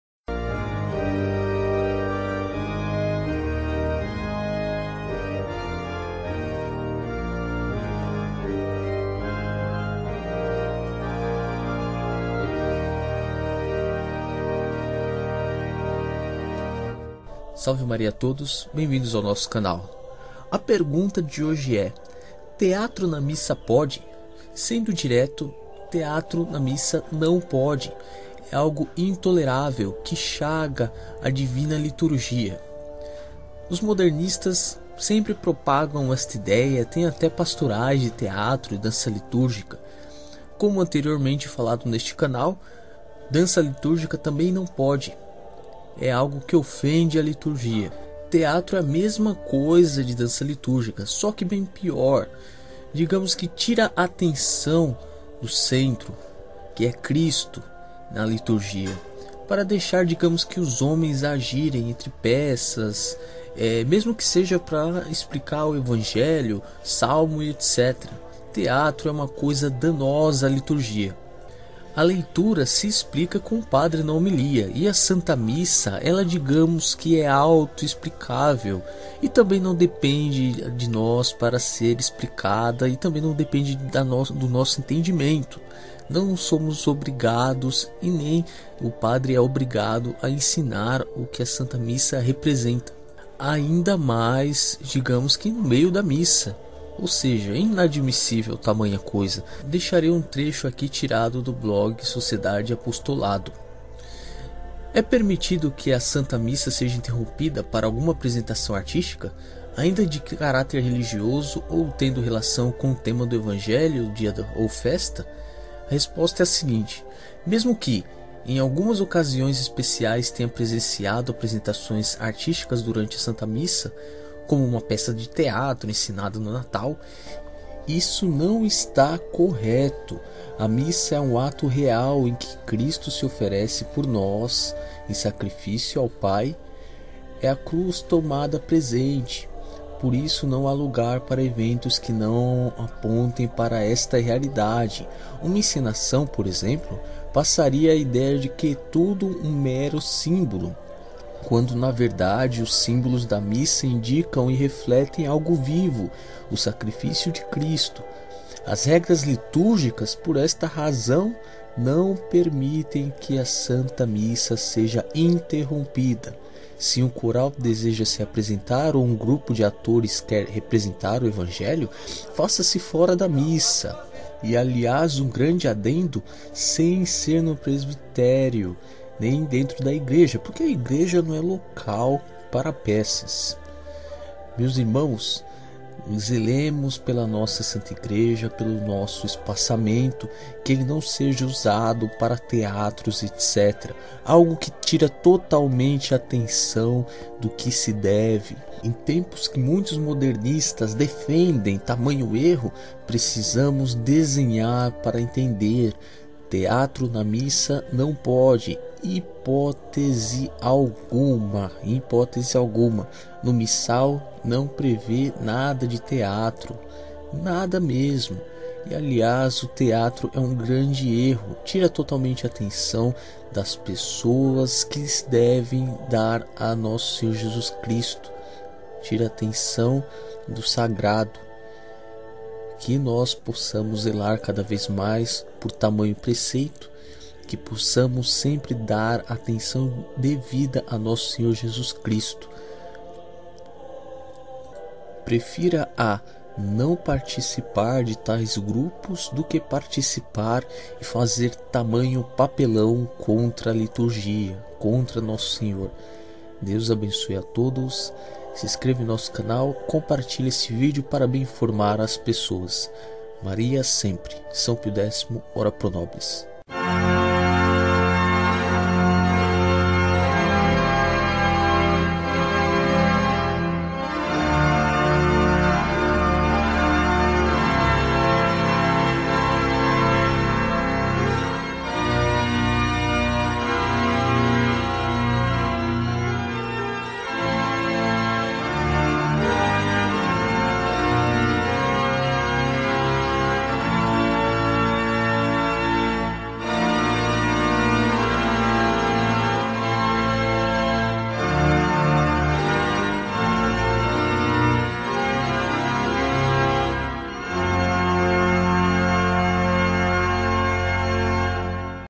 TEATRO NA MISSA NÃO PODE! ABUSO LITÚRGICO!
Atenção! Teatro na Missa NÃO PODE EM HIPOTESE ALGUMA! Modernistas sempre a propagar erros...